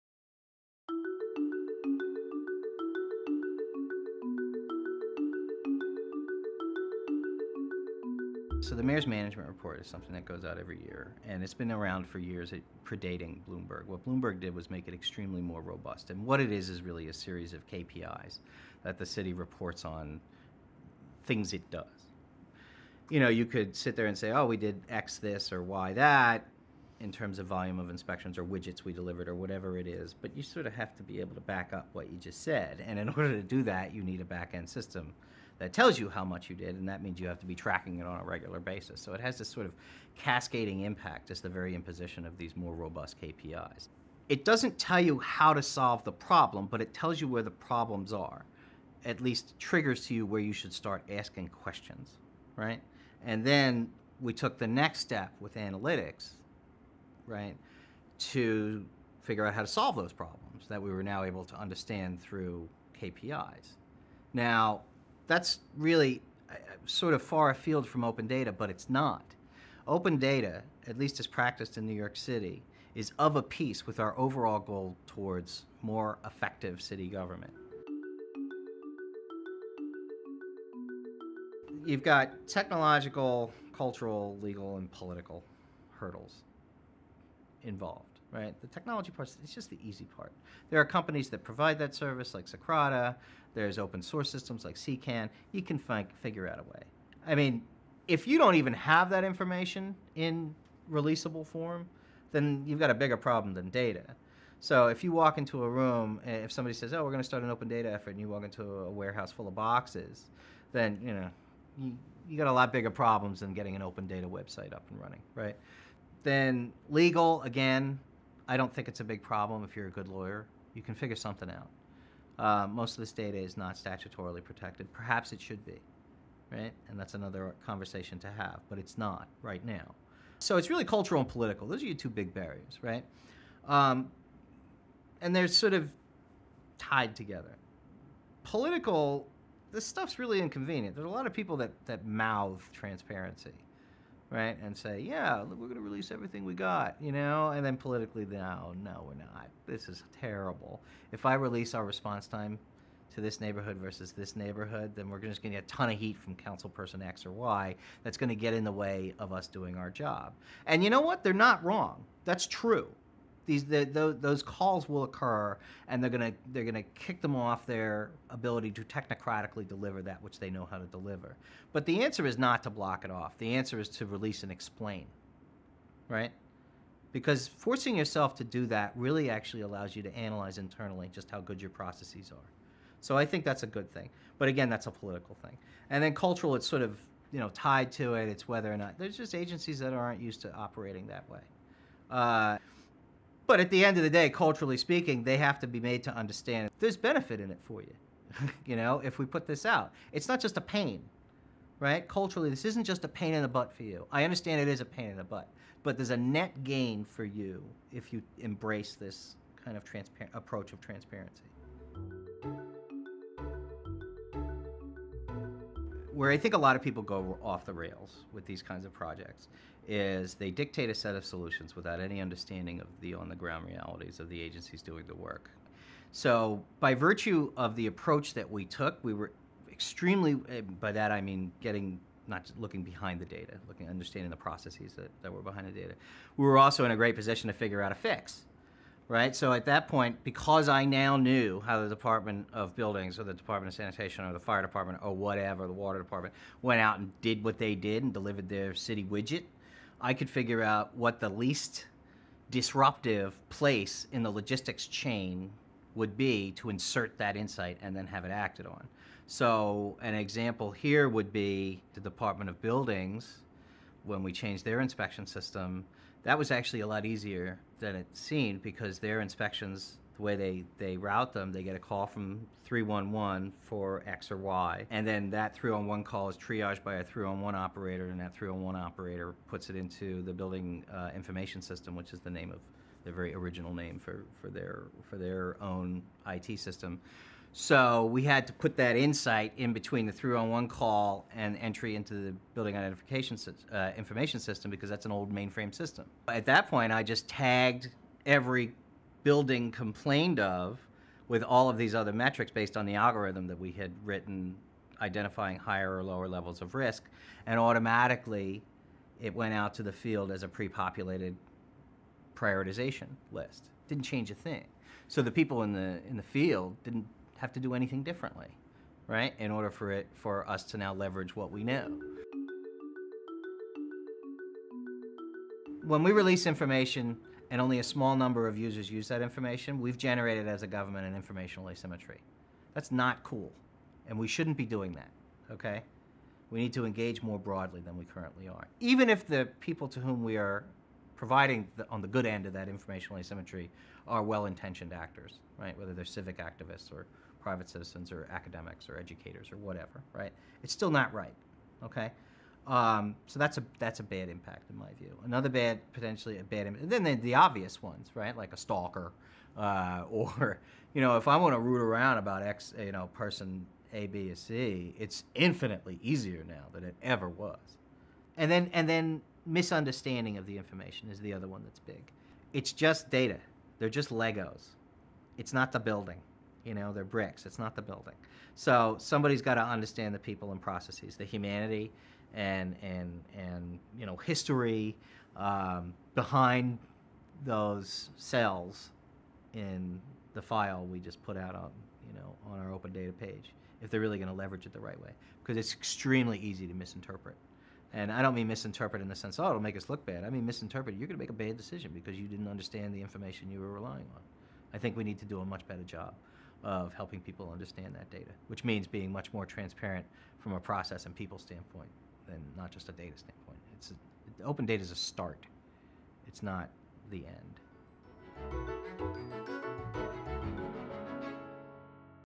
Learning from New York City's open-data effort
Finding the technology needed to make data truly open is easy. Breaking down barriers to change and helping users understand the information takes work.

In this interview with McKinsey Global Institute partner Michael Chui, the city's former chief analytics officer Mike Flowers details the bridges that needed to be built, internally and externally, to make New York's open-data effort succeed.

An edited transcript of the his remarks are available on our